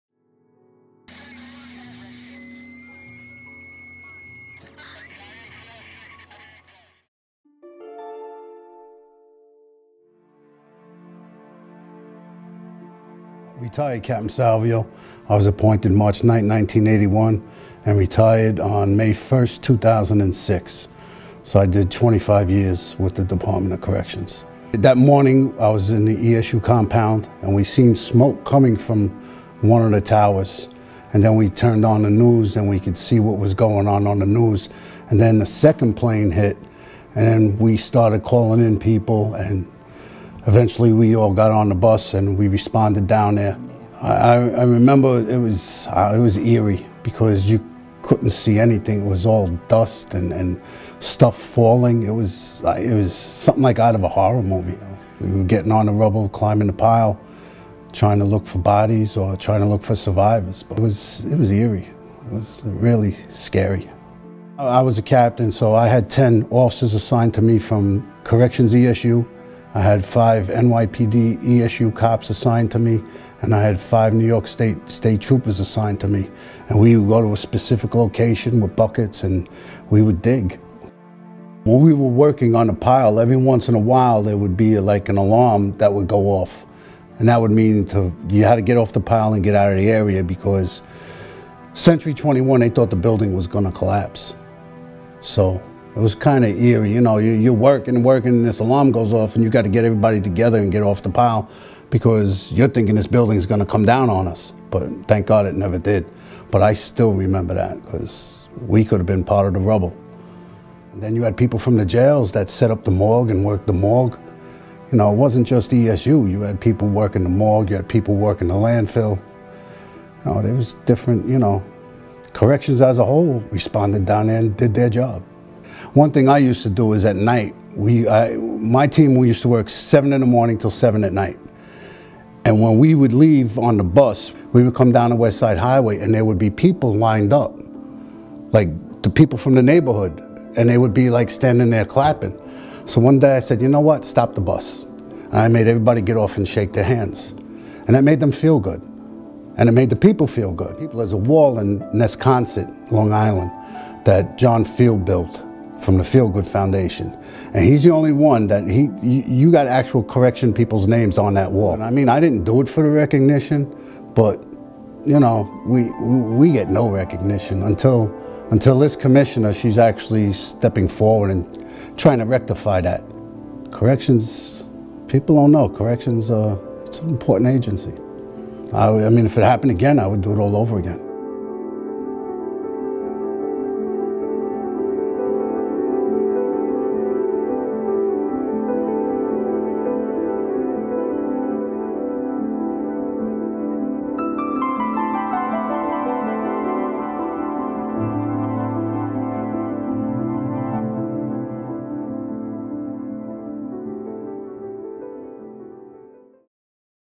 We Were There: Captain Salvio
Hear Captain Salvio's account of his experience at Ground Zero.